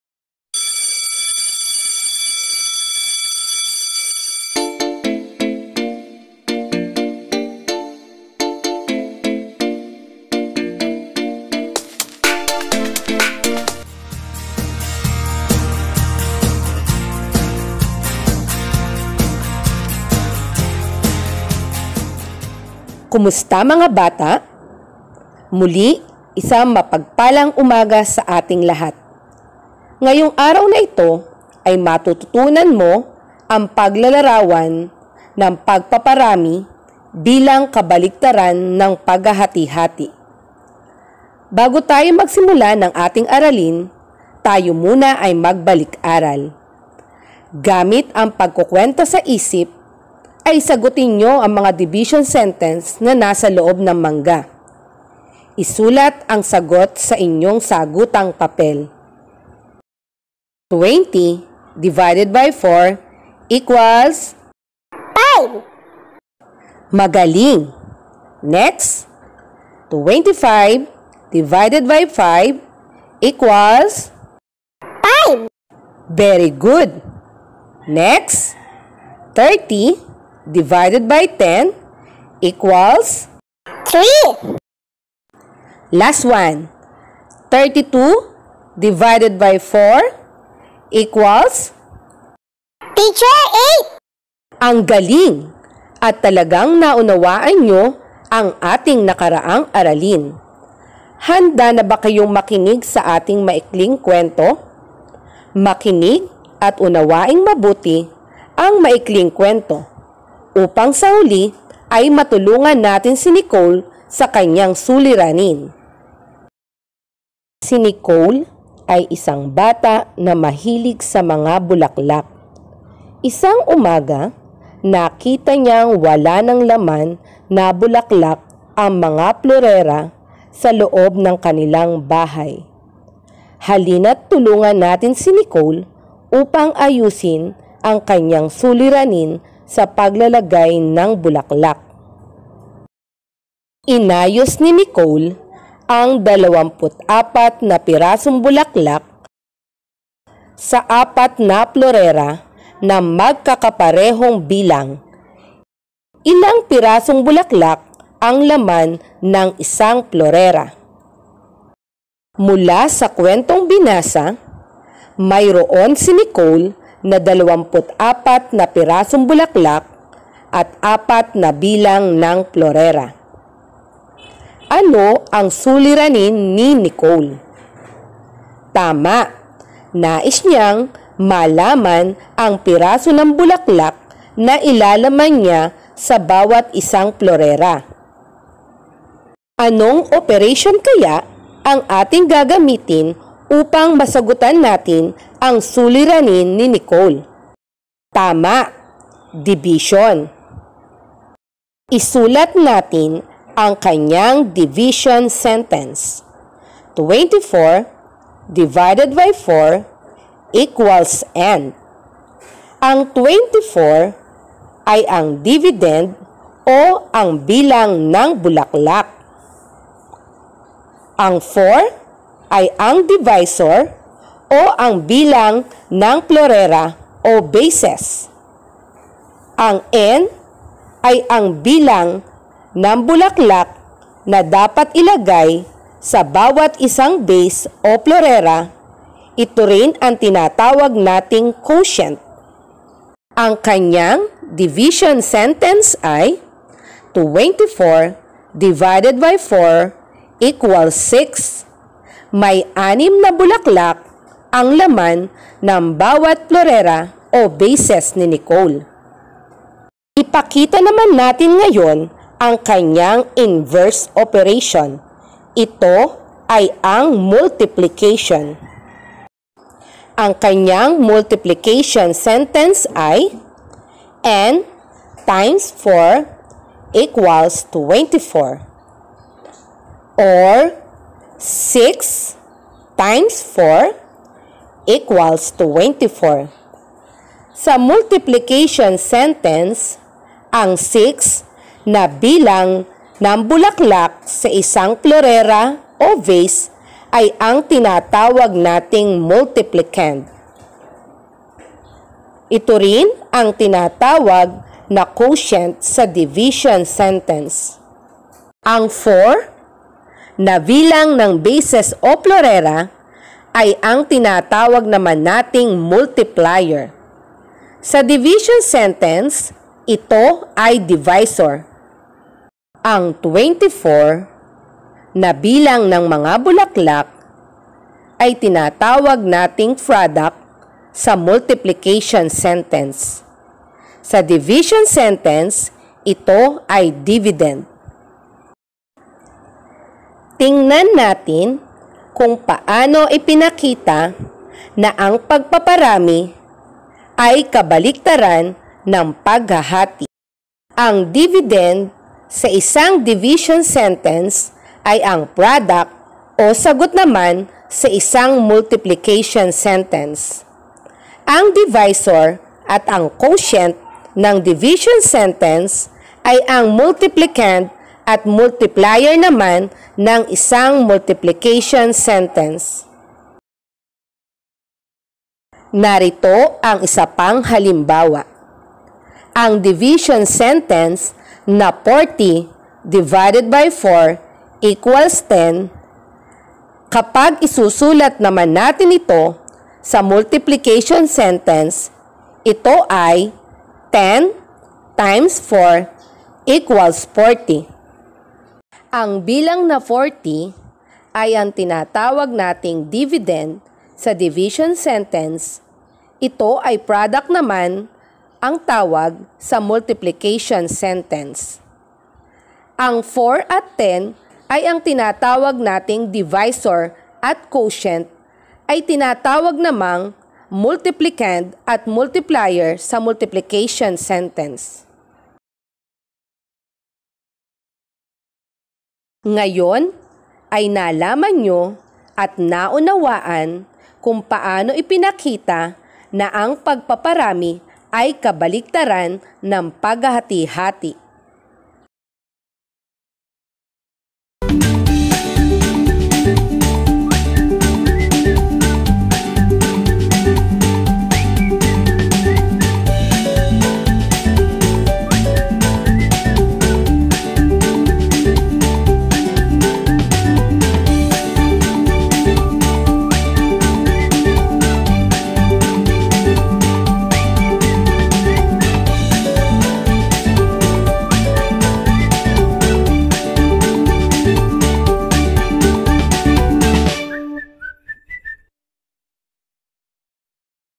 Mathematics 2 (Q3-Week 3) Paglalarawan ng Pagpaparami at Paghahati-hati Bilang Inverse Operations
Ang mga larawan na inyong makikita ay kuha sa internet ito ay hindi pag aari ng guro.Ctto sa owner ng mga larawan